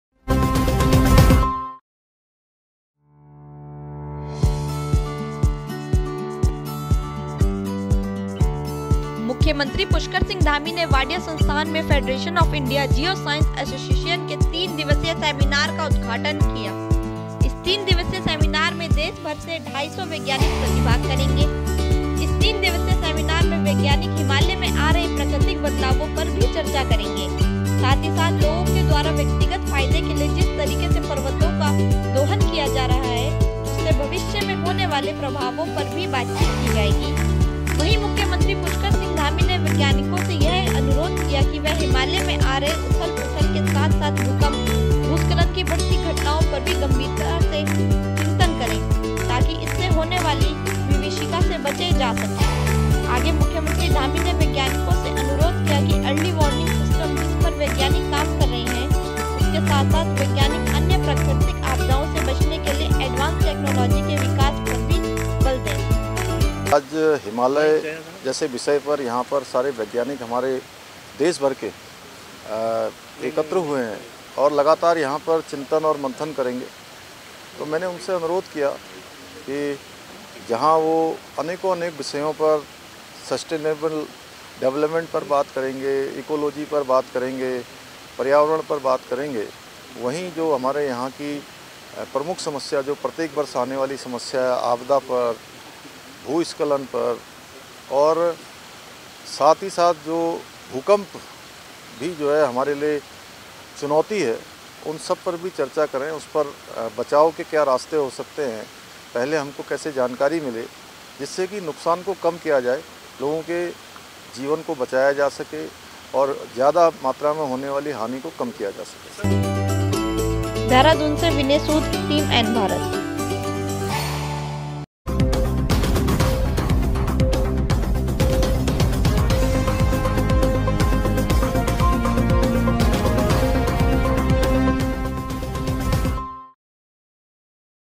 देहरादून || सीएम धामी ने तीन दिवसीय सेमिनार का उद्घाटन किया || N भारत
सीएम धामी ने तीन दिवसीय सेमिनार का उद्घाटन किया || N भारत

मुख्यमंत्री पुष्कर सिंह धामी ने वाडिया संस्थान में फेडरेशन ऑफ इंडिया जियो साइंस एसोसिएशन के तीन दिवसीय सेमिनार का उद्घाटन किया।